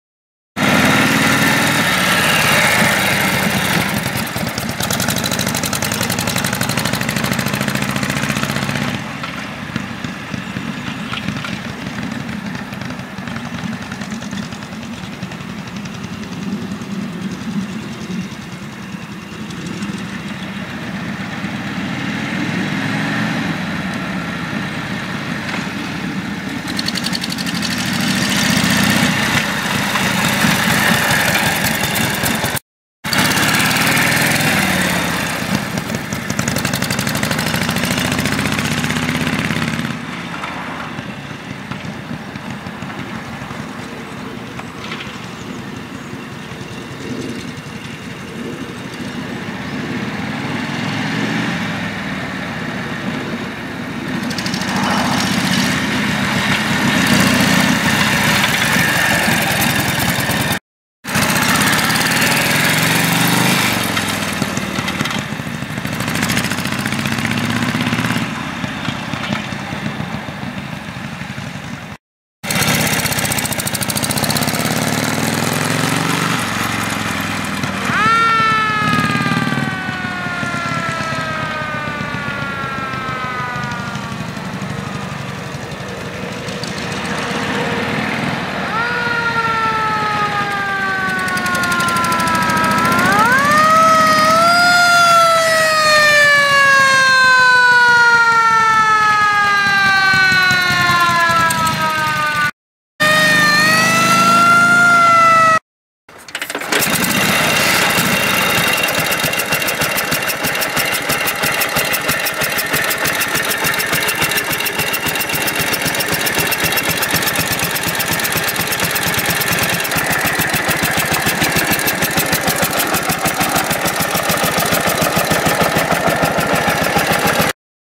Harley Davidson WLA 750cc from 1942
Test drive of the Harley Davidson WLA 750cc from 1942.